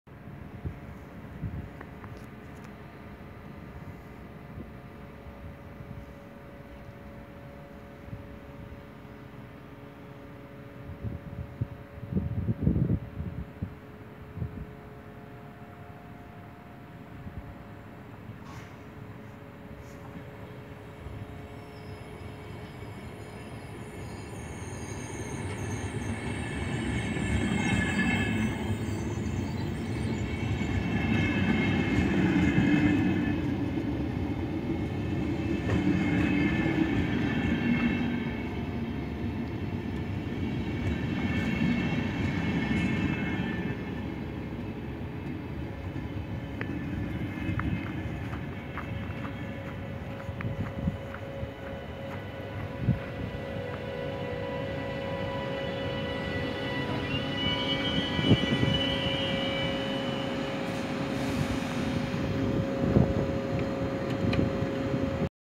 IC 944 Turku-Helsinki arrives to Helsinki Central station
Edo ohjausvaunullinen Intercity juna 944 saapuu Helsingin päärautatieasemalle